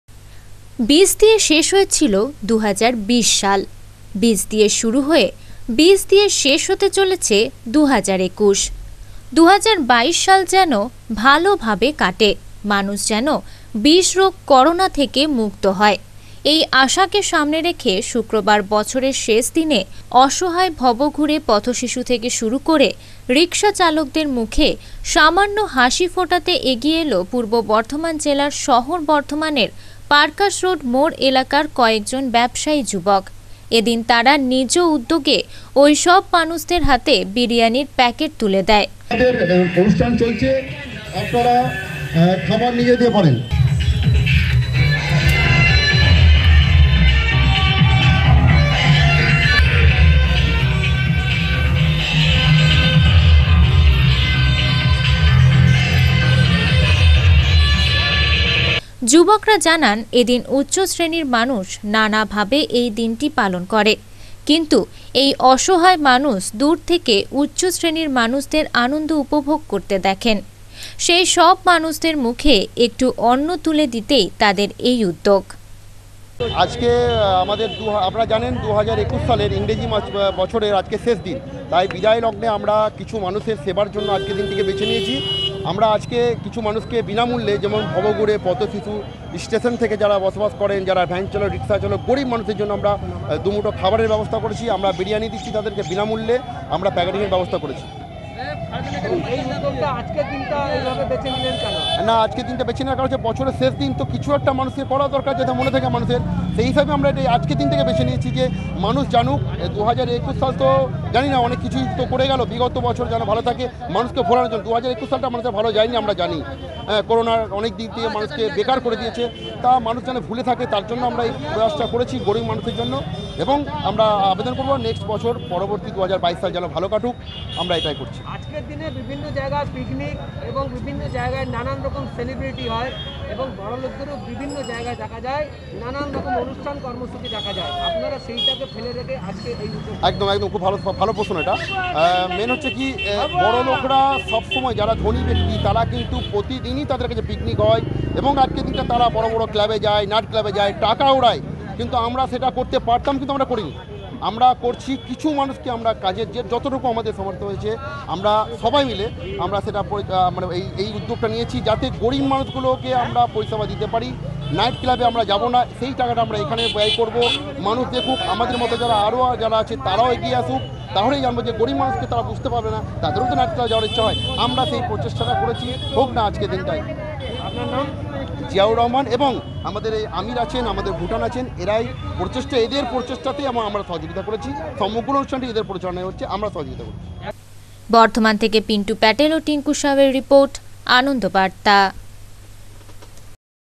ভবঘুরে মানুষ ও রিকশাচালকদের 2022 এর আগে 2021 শেষ দিনে বিরিয়ানি খাবার বিতরণ করা হয়
পূর্ব বর্ধমান শহর বর্ধমানের পারকাস রোড এলাকায় ভবঘুরে মানুষ ও রিকশাচালকদের 2022 এর আগে 2021 শেষ দিনে বিরিয়ানি খাবার বিতরণ করা হয়